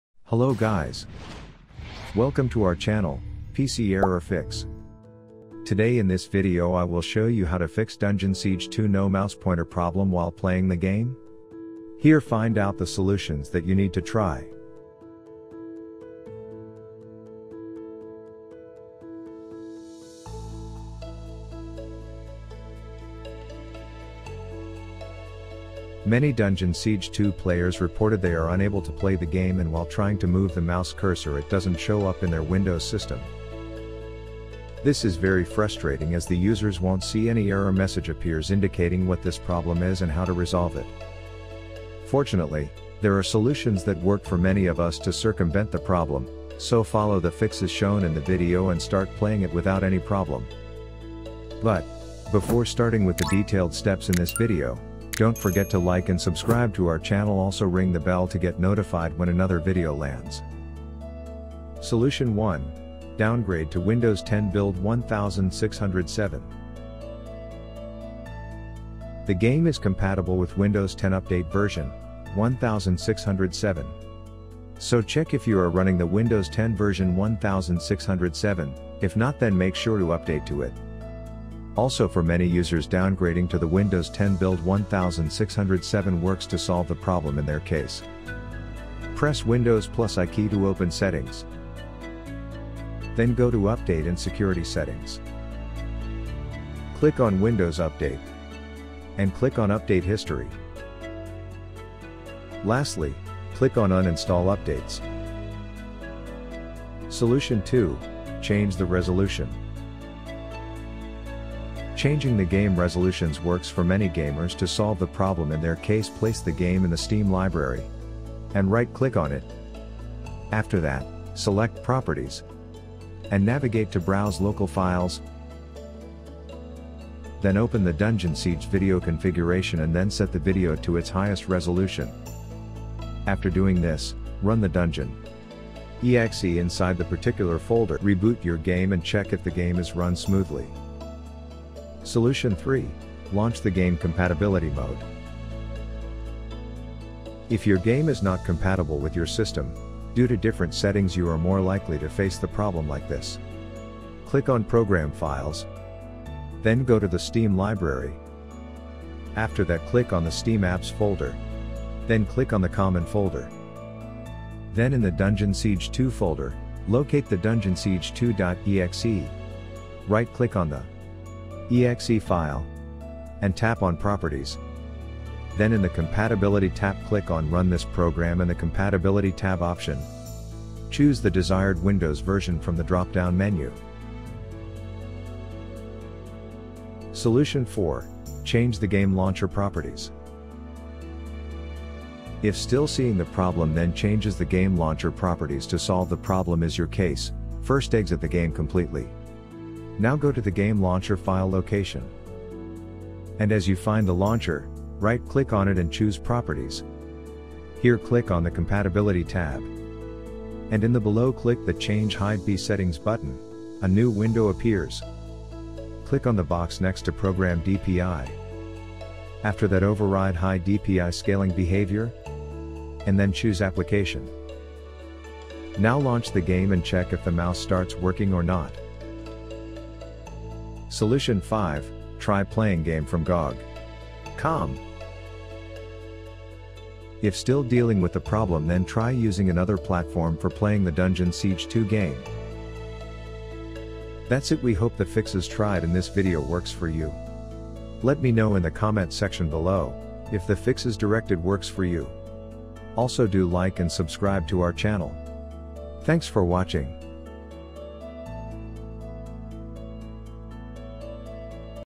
How to Fix Dungeon Siege 2 No Mouse Pointer? [100% Resolved]
DungeonSiege2 no mouse pointer is an irritating error as per the users while trying to move the mouse cursor it doesn’t show up in their Windows system and were unable to play the game. The problem is related to the game you are playing or the system you’re using to play the game or the in-game resolutions issues.

This needed to be fixed ASAP, so watch the video to know how to fix  Dungeon Siege 2 no mouse pointer problem.

Key Moments

0.02 Intro
1:02 Solution 1  – Downgrade to Windows 10 Build 1607
1:50 Solution 2 – Change the Resolution
2:24 Solution 3 – Launch the Game Compatibility Mode
3:09 Solution 4 – Changing the Game Launcher Properties
3:51 Solution 5 – Try Playing Game from Gog.com